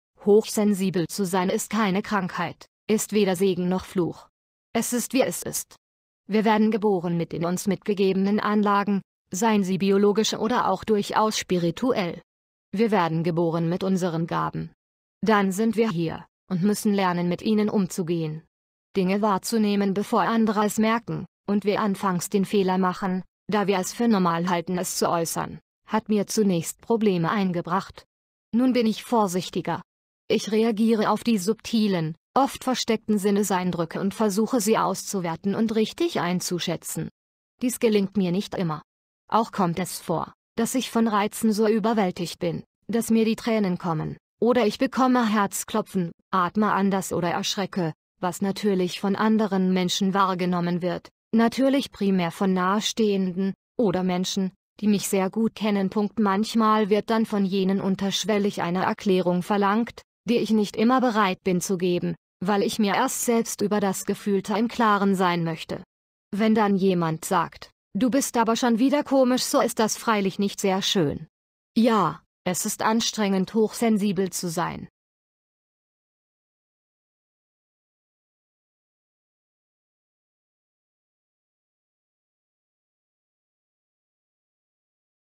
Hochsensibel
Hochsensibel zu sein ist keine Krankheit, ist weder Segen noch Fluch. Es ist wie es ist. Wir werden geboren mit den uns mitgegebenen Anlagen; seien sie biologisch oder auch durchaus spirituell. Wir werden geboren mit unseren Gaben. Dann sind wir hier, und müssen lernen mit ihnen umzugehen. Dinge wahrzunehmen bevor andere es merken, und wir anfangs den Fehler machen, da wir es für normal halten es zu äußern, hat mir zunächst Probleme eingebracht. Nun bin ich vorsichtiger.
Ich reagiere auf die subtilen, oft versteckten Sinneseindrücke und versuche sie auszuwerten und richtig einzuschätzen. Dies gelingt mir nicht immer. Auch kommt es vor, dass ich von Reizen so überwältigt bin, dass mir die Tränen kommen, oder ich bekomme Herzklopfen, atme anders oder erschrecke, was natürlich von anderen Menschen wahrgenommen wird; natürlich primär von Nahestehenden, oder Menschen, die mich sehr gut kennen.Manchmal wird dann von jenen unterschwellig eine Erklärung verlangt, die ich nicht immer bereit bin zu geben, weil ich mir erst selbst über das Gefühlte im Klaren sein möchte. Wenn dann jemand sagt, „ du bist aber schon wieder komisch“ so ist das freilich nicht sehr schön. Ja, es ist anstrengend hochsensibel zu sein !

Hartmut Alt